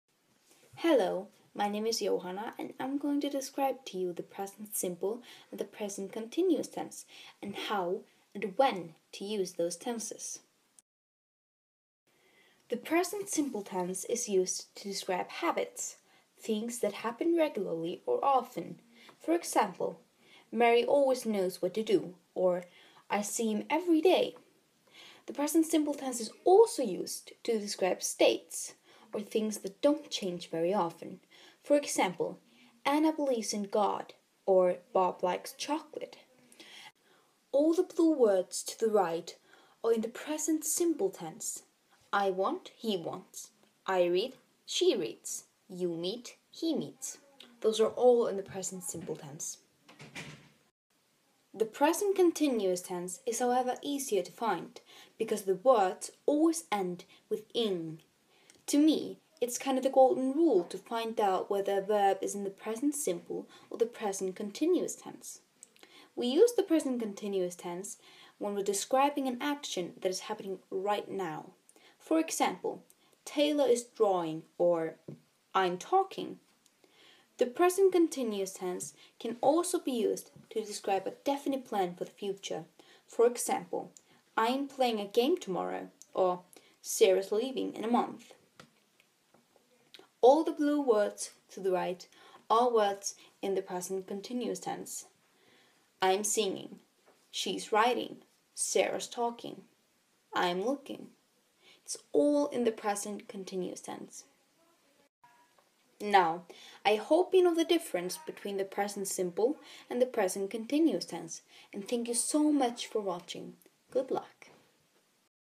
Einföld og samsett nútíð - Present simple and continuous
Jóhanna Ingisól Sævarsdóttir, nemandi í 9. Bekk, fjallar um einfalda og samsetta nútíð í enskum sögnum.  Hún útskýrir með dæmum notkun tíðanna og muninn á þeim tveimur.